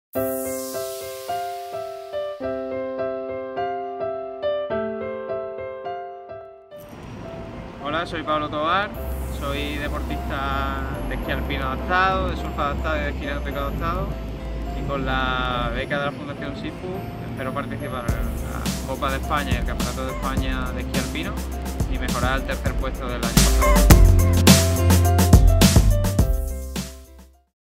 El reto de Pablo Tovar - II Becas de Apoyo al Deporte Adaptado
La Fundación Grupo SIFU ha otorgado en 2016 las II Becas de Apoyo al Deporte Adaptado y Pablo Tovar ha sido uno de los premiados.

Pablo lucha por ser campeón de España de Esquí Adaptado.